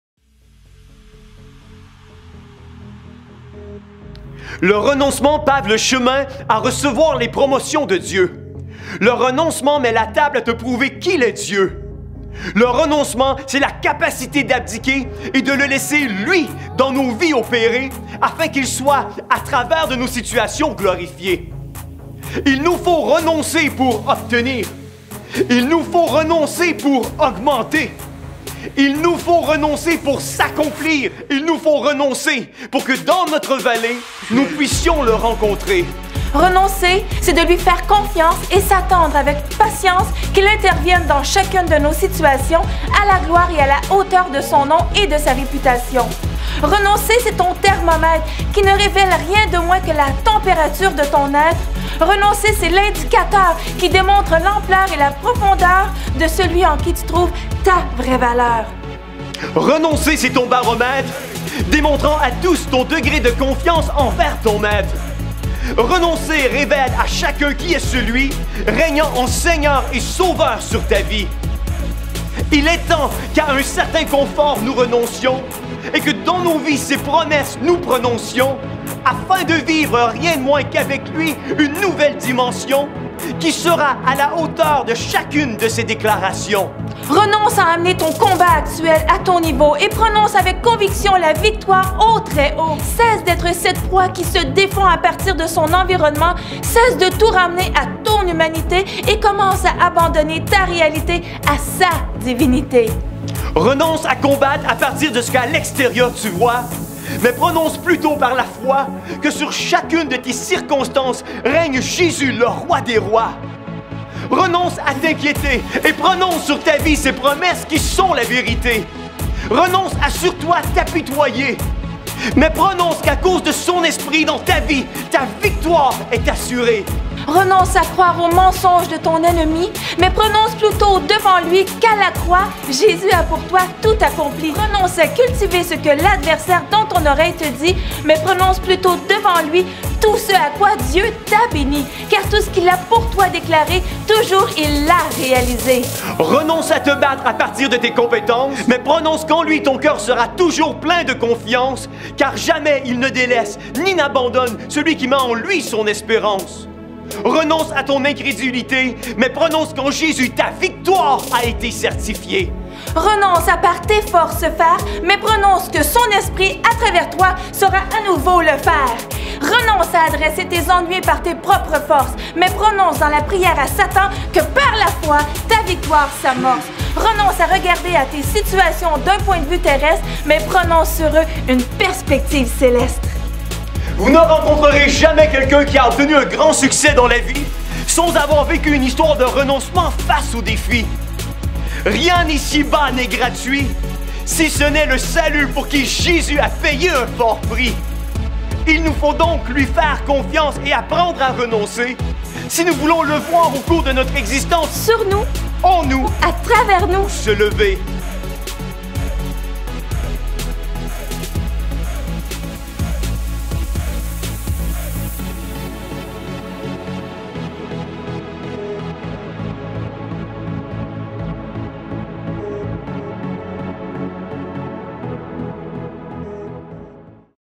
SLAM - Le Renoncement
Il nous faut donc lui faire confiance et apprendre à renoncer !

Si nous voulons le voir au cours de notre existence sur nous, en nous et, à travers nous Se lever !

Bonne écoute !!

L'équipe ThéoVox !